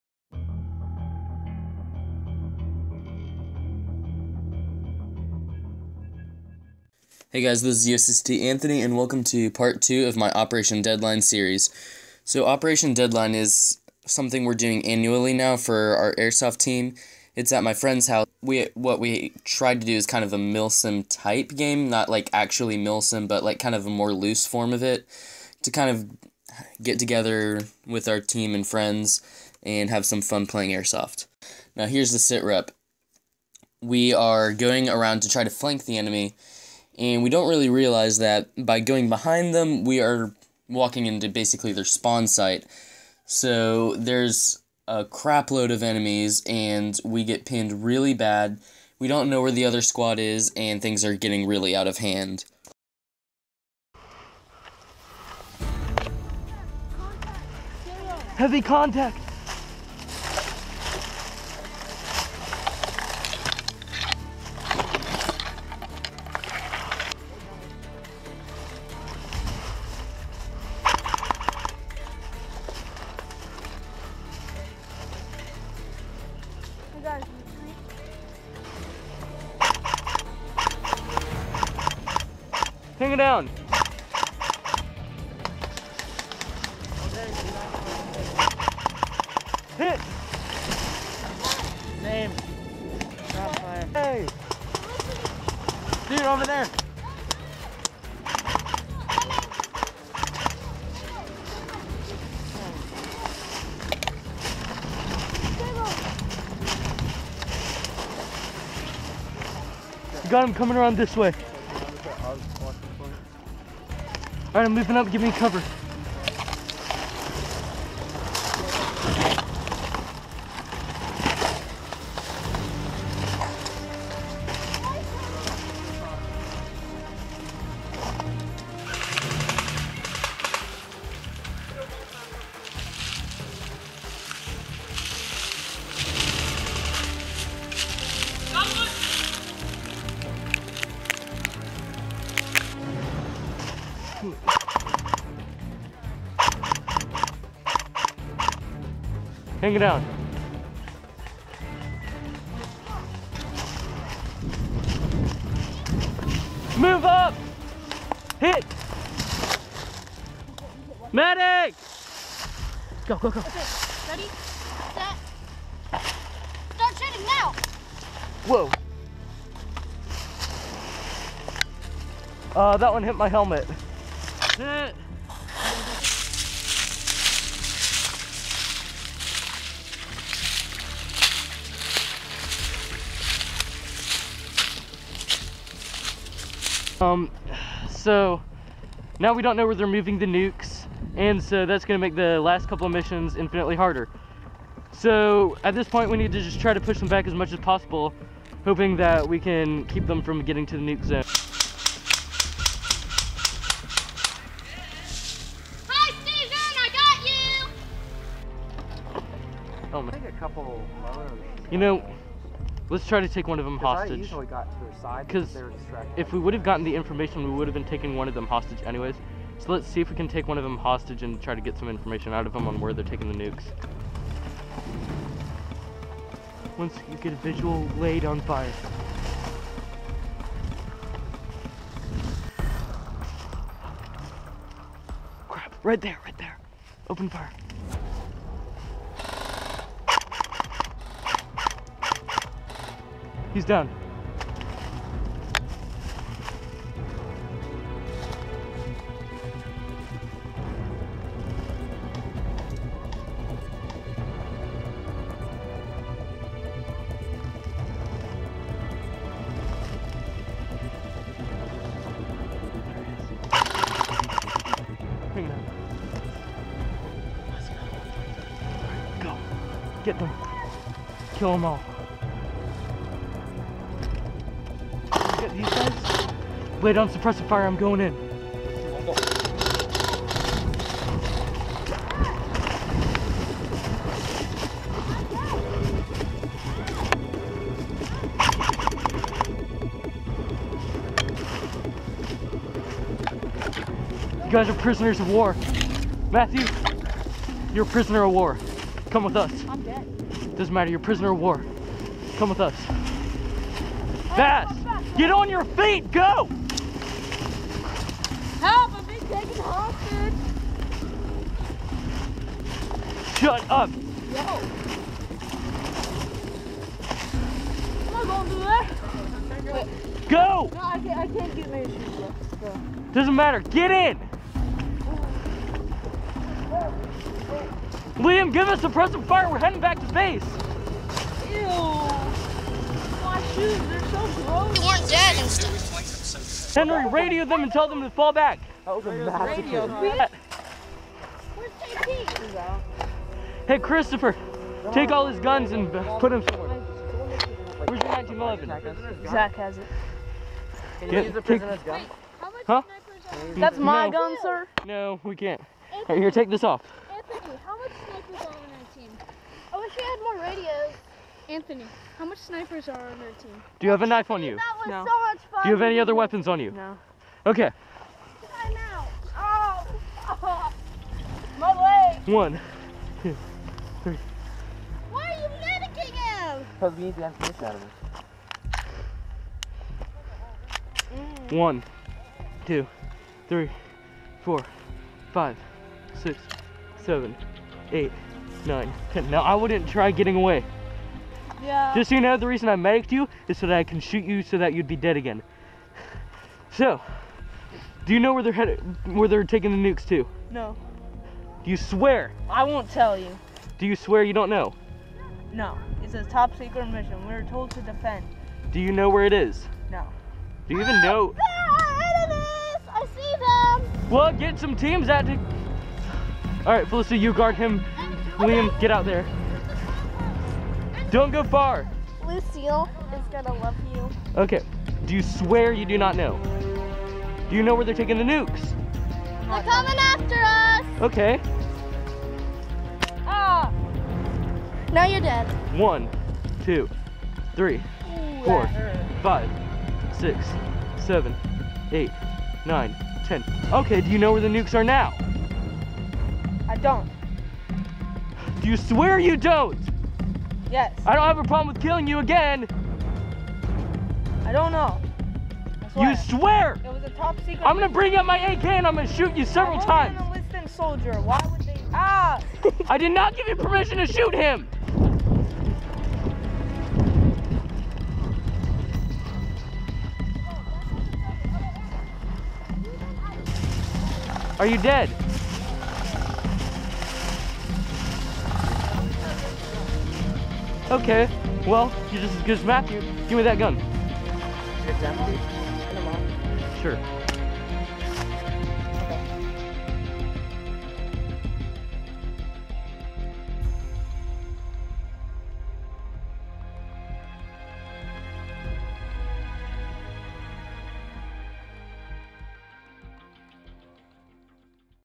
Operation Deadline Part 2: Prisoner of War
Part 2 of Operation Deadline, a private 2 hour op I hosted for my team! In this episode, we decide to take prisoners to figure out what the Soviet's plan was regarding the nukes.